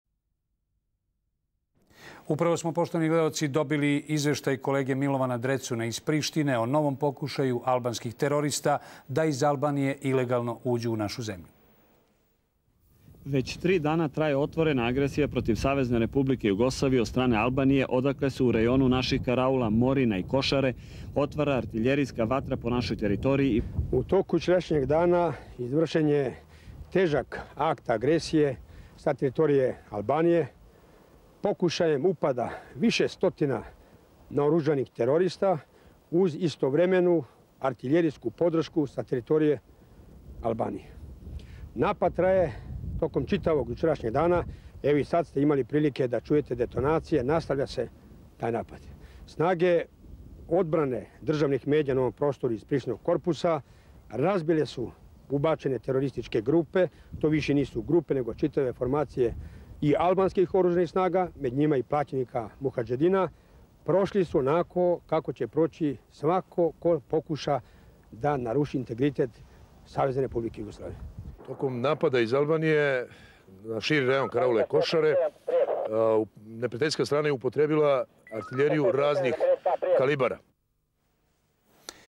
Агресија НАТО на СР Југославију: рејон карауле "Кошаре" 10. април 1999.
почетак копнене агресије, село Батуша, рејон карауле "Кошаре" 10. април 1999.